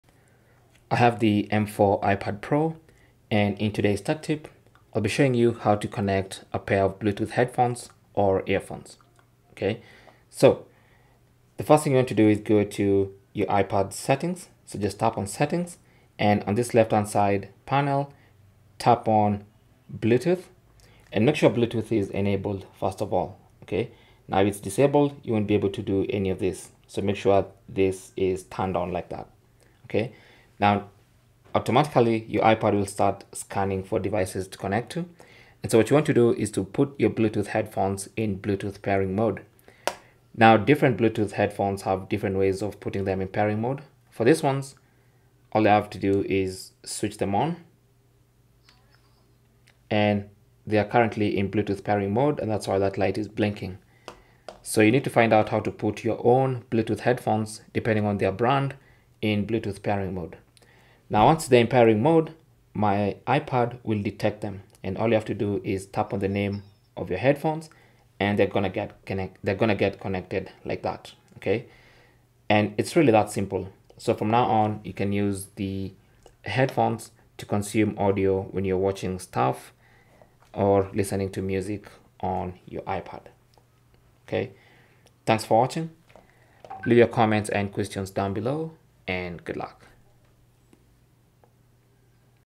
How To Connect Bluetooth Headphones On M4 iPad Pro
Learn How To Connect Bluetooth Headphones On M4 iPad Pro.

In the era of wireless technology, Bluetooth headphones have become a staple for many users looking to enjoy their favorite music, podcasts, or videos without the hassle of cords. If you’re the proud owner of an iPad Pro M4 and you're wondering how to pair your Bluetooth headphones with it, you've come to the right place.

This guide will walk you through the simple steps to connect your Bluetooth headphones to your iPad Pro M4 effortlessly.

1: First things first, ensure that your Bluetooth headphones are turned on and are in pairing mode. Typically, this involves holding down the power button for a few seconds until you see a blinking light indicating that the headphones are ready to pair.

2: Unlock your iPad Pro M4 and navigate to the home screen. Locate and tap on the “Settings” icon, which looks like a gear. Within the Settings menu, find and tap on “Bluetooth.” This will open the Bluetooth settings menu where you can manage all Bluetooth connections on your iPad Pro M4.

3: If Bluetooth is not already enabled, toggle the switch to turn it on. Your iPad Pro M4 will start searching for nearby Bluetooth devices. Once your Bluetooth headphones appear in the list of available devices on your iPad Pro M4, tap on them to initiate the pairing process.

4: Your iPad Pro M4 will establish a connection with your Bluetooth headphones. You’ll typically hear a confirmation sound in your headphones, and the Bluetooth settings menu on your iPad Pro M4 will display “Connected” next to your headphones' name.

Apple iPad Pro 13-Inch (M4):

Apple iPad Pro 11-Inch (M4):

Cell Phone Tripod Adapter